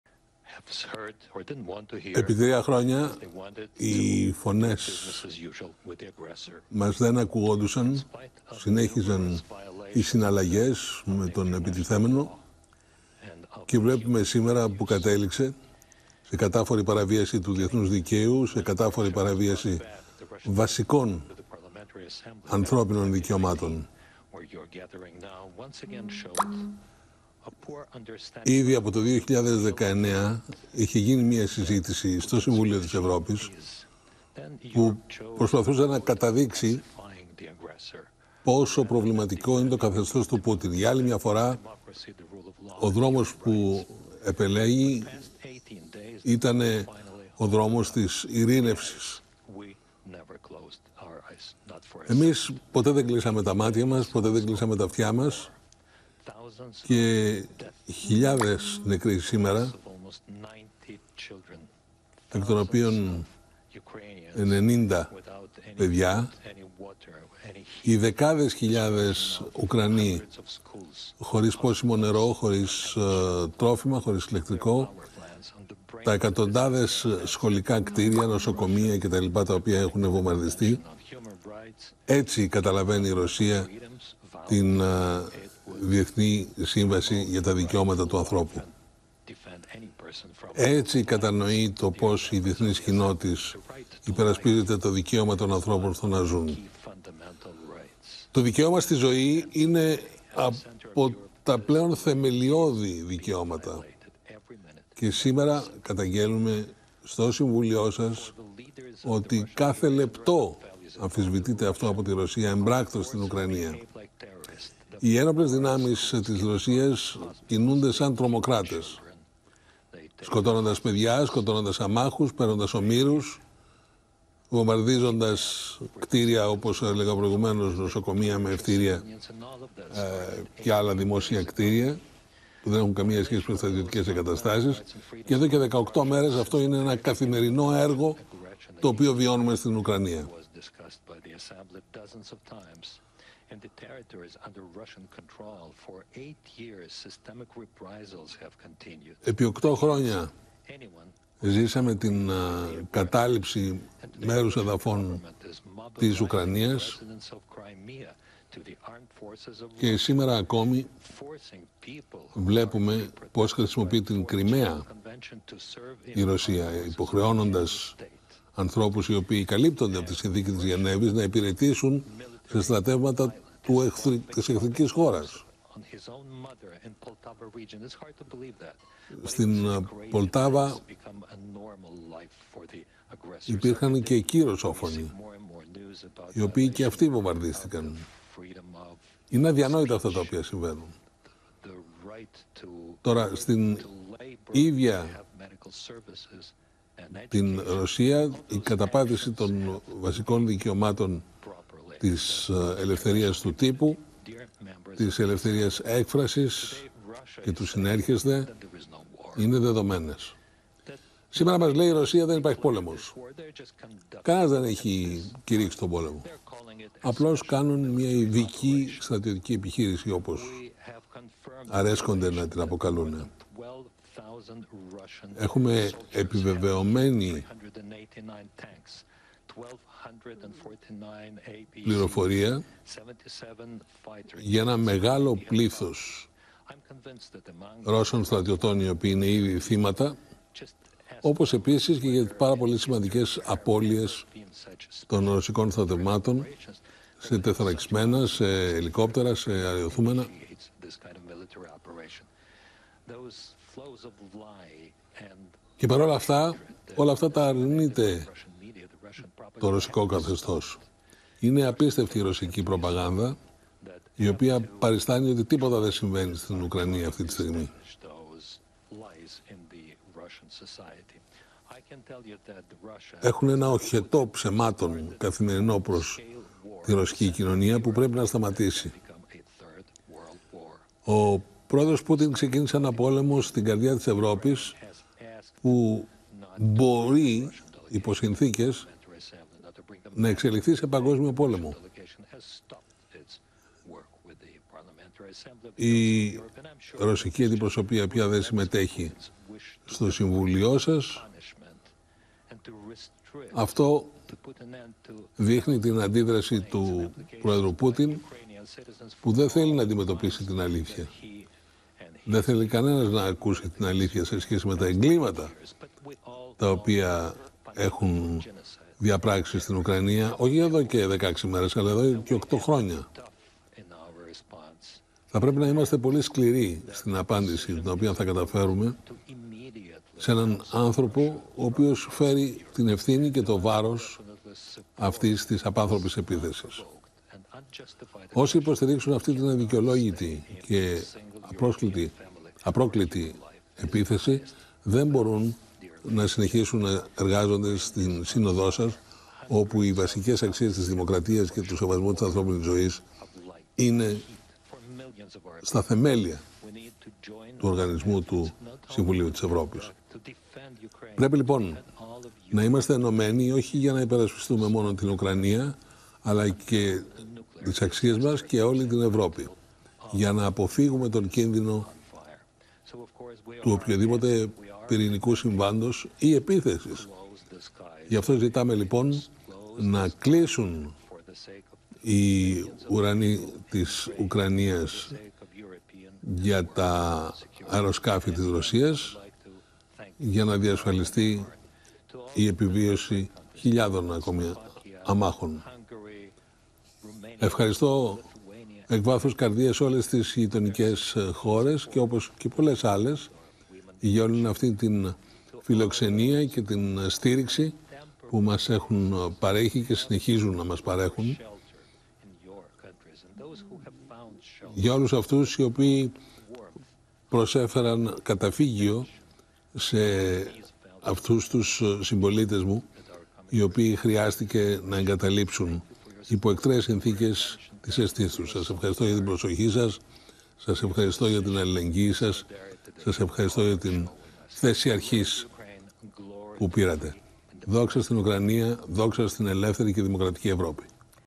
Πρωθυπουργός Ουκρανίας: Οχετός ψεμάτων από τη ρωσική προπαγάνδα | 14/3/2022 | OPEN TV
Γίνετε μέλος στο κανάλι του OPEN για να αποκτήσετε πρόσβαση σε προνόμια:
«Οι Ρώσοι χτυπούν νοσοκομεία, σχολεία και αμάχους. Μεγάλος αριθμός Ρώσων στρατιωτών είναι νεκροί. Οχετός ψεμάτων από τη ρωσική προπαγάνδα. Ο πόλεμος στην Ουκρανία μπορεί να εξελιχθεί σε Γ’ Παγκόσμιο», δήλωσε μεταξύ άλλων ο Πρωθυπουργός της Ουκρανίας.